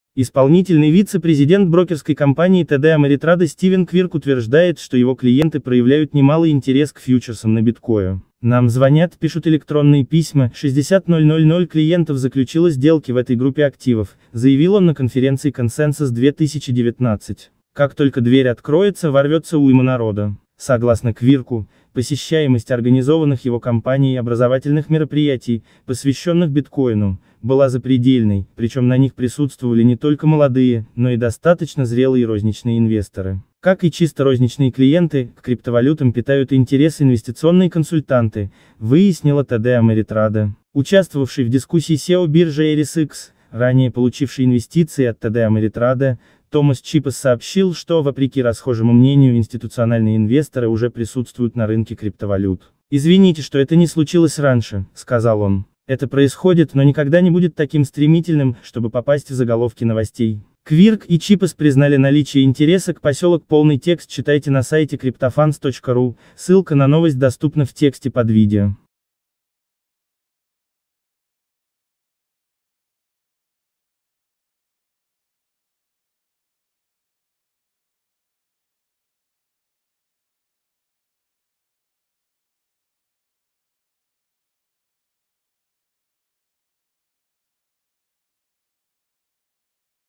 Вице-президент td Ameritrade: Десятки тысяч наших клиентов питают интерес к криптовалютам
Исполнительный вице-президент брокерской компании TD Ameritrade Стивен Квирк утверждает, что его клиенты проявляют немалый интерес